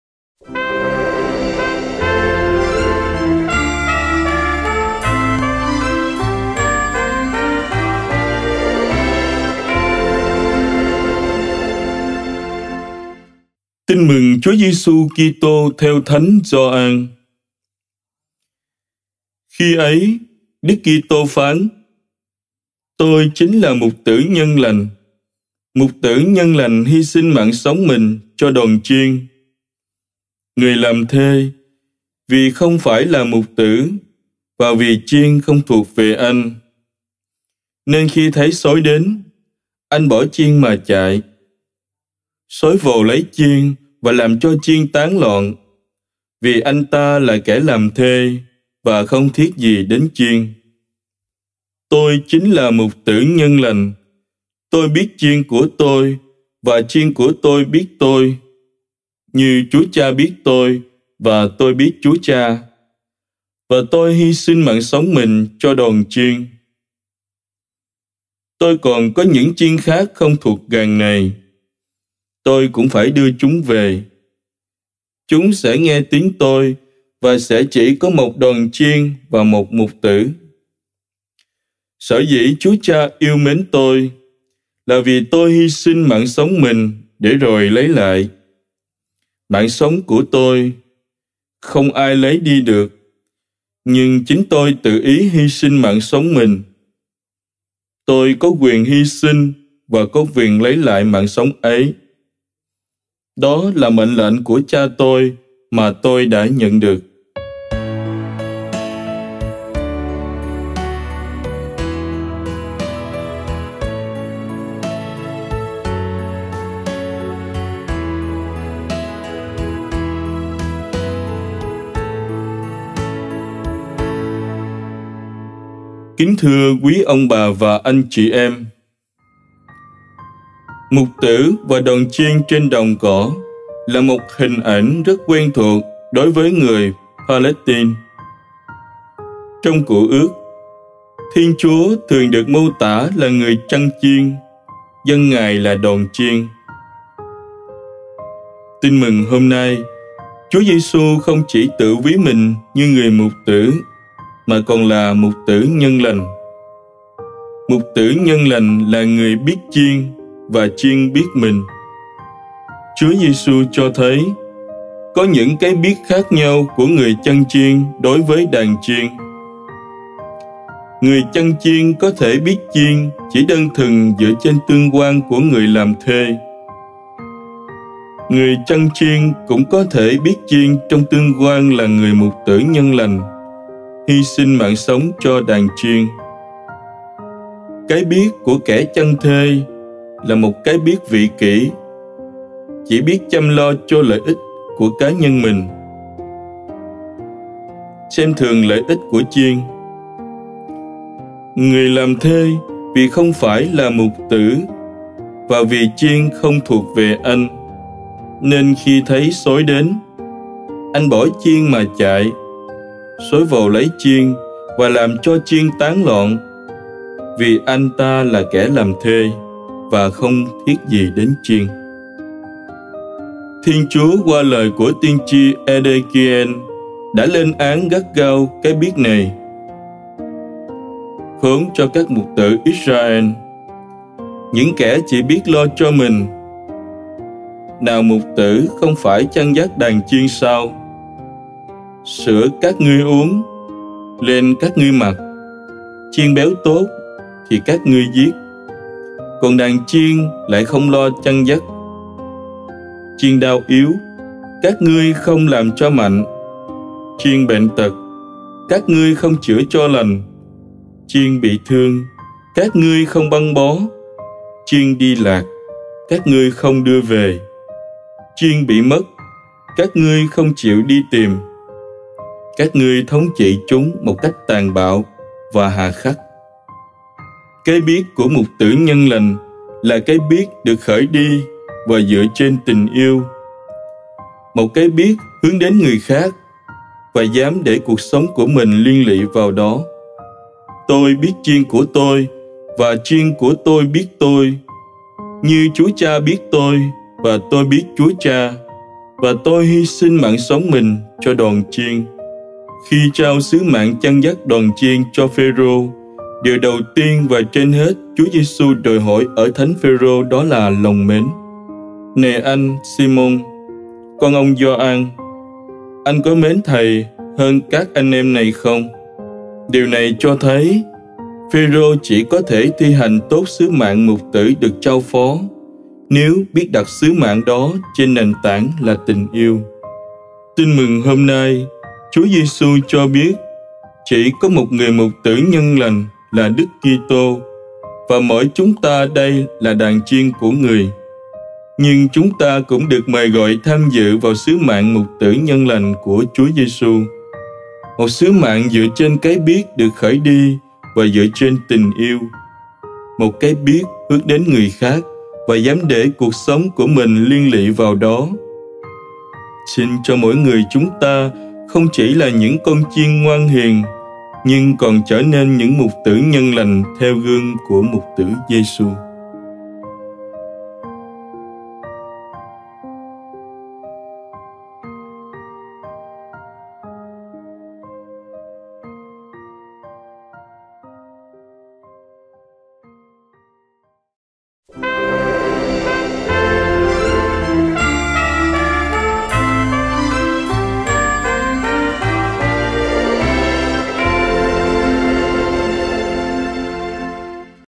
Suy Niệm Lời Chúa: Chúa Nhật IV Phục Sinh - Năm B, 25/04/2021
Chúa Nhật IV Phục Sinh - Năm B: Xin Chúa cho mỗi người chúng ta không chỉ là những con chiên ngoan hiền, nhưng còn trở nên những mục tử nhân lành theo gương của Mục tử Giêsu.
Mục tử và đoàn chiên trên đồng cỏ là một hình ảnh rất quen thuộc đối với người Palestina. Trong Cựu ước, Thiên Chúa thường được mô tả là người chăn chiên, dân Ngài là đoàn chiên (x. Tv 17,45; 19,13; 23,1; 30,1; 77,20; 95,7; 100,3; Is 40,11). Tin Mừng hôm nay, Chúa Giêsu không chỉ tự ví mình như người mục tử, mà còn là mục tử nhân lành. Mục tử nhân lành là người biết chiên, và chiên biết mình.
Chúa Giêsu cho thấy, có những cái biết khác nhau của người chăn chiên đối với đàn chiên. Người chăn chiên có thể biết chiên chỉ đơn thuần dựa trên tương quan của người làm thuê. Người chăn chiên cũng có thể biết chiên trong tương quan là người mục tử nhân lành hy sinh mạng sống cho đàn chiên.
Cái biết của kẻ chăn thuê là một cái biết vị kỷ - chỉ biết chăm lo cho lợi ích của cá nhân mình, xem thường lợi ích của chiên. “Người làm thuê, vì không phải là mục tử, và vì chiên không thuộc về anh, nên khi thấy sói đến, anh bỏ chiên mà chạy. Sói vồ lấy chiên và làm cho chiên tán loạn, vì anh ta là kẻ làm thuê, và không thiết gì đến chiên”. Thiên Chúa qua lời của tiên tri Edêkien đã lên án gắt gao cái biết này: “Khốn cho các mục tử Ítrael, những kẻ chỉ biết lo cho mình! Nào mục tử không phải chăn dắt đàn chiên sao? Sữa các ngươi uống, len các ngươi mặc, chiên béo tốt thì các ngươi giết, còn đàn chiên lại không lo chăn dắt. Chiên đau yếu, các ngươi không làm cho mạnh; chiên bệnh tật, các ngươi không chữa cho lành; chiên bị thương, các ngươi không băng bó; chiên đi lạc, các ngươi không đưa về; chiên bị mất, các ngươi không chịu đi tìm. Các ngươi thống trị chúng một cách tàn bạo và hà khắc” (Ed 34,2-4).
Cái biết của mục tử nhân lành là cái biết được khởi đi và dựa trên tình yêu. Một cái biết hường đến người khác và dám để cuộc sống của mình liên lụy vào đó. “Tôi biết chiên của tôi, và chiên của tôi biết tôi, như Chúa Cha biết tôi, và tôi biết Chúa Cha, và tôi hy sinh mạng sống mình cho đoàn chiên”..
Khi trao sứ mạng chăn dắt đoàn chiên cho Phê-rô, điều đầu tiên và trên hết Chúa Giêsu đòi hỏi ở thánh Phê-rô đó là lòng mến. “Này anh Si-môn, con ông Gio-an, anh có mến Thầy hơn các anh em này không ?”. (x. Ga 21,15-17). Điều này cho thấy, Phêrô chỉ có thể thi hành tốt sứ mạng mục tử được trao phó nếu biết đặt sứ mạng đó trên nền tảng là tình yêu.
Tin Mừng hôm nay, Chúa Giêsu cho biết: Chỉ có một người mục tử nhân lành là Đức Kitô và mỗi chúng ta đây là đàn chiên của Người. Nhưng chúng ta cũng được mời gọi tham dự vào sứ mạng mục tử nhân lành của Chúa Giêsu. Một sứ mạng dựa trên cái biết được khởi đi và dựa trên tình yêu. Một cái biết hướng đến người khác và dám để cuộc sống của mình liên lụy vào đó. Xin Chúa cho mỗi người chúng ta không chỉ là những con chiên ngoan hiền, nhưng còn trở nên những mục tử nhân lành theo gương của Mục tử Giêsu.